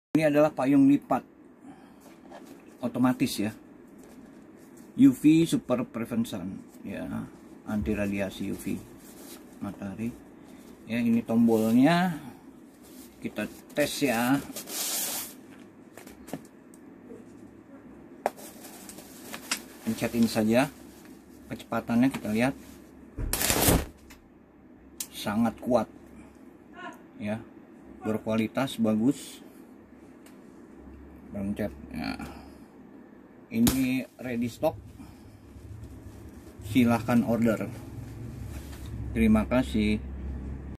Payung Otomatis Buka Tutup Anti UV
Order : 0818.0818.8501

Payung otomatis ini apabila ditekan tombolnya dapat terbuka dengan sendirinya. Lalu pada saat payung terbuka, apabila tombol yang sama ditekan kembali maka payung akan bisa menutup sendiri. Praktis, cocok untuk kamu yang ingin kemudahan dan kecepatan pada waktu hujan. Menghemat waktu membuka payung berarti lebih kecil kemungkinan kamu akan terkena guyuran hujan.

Ada banyak tipe otomatis di pasaran, yang ini grade super alias paling bagus otomatisnya. Kelemahan payung otomatis ini sekali mekanisme otomatisnya rusak, payung ga bisa terpakai. Jadi lebih bijak beli yang kualitas Super dan Awet, daripada beli yang otomatis yang kurang baik.

Berat: 500 gram
Ukuran panjang : 28 cm (kondisi dilipat)
Diameter: 98 cm
panjang kondisi dilipat: 28 cm
panjang kondisi ditarik: 58 cm
Lipat 3
Jari2 : 8

Tombol otomatis itu untuk memanjangkan payung, membuka dan menutu kembali payung. Untuk dipendekan kembali harus manual dengan tangan. Apabila payung masih baru, memang agak keras untuk dipendekan kembali.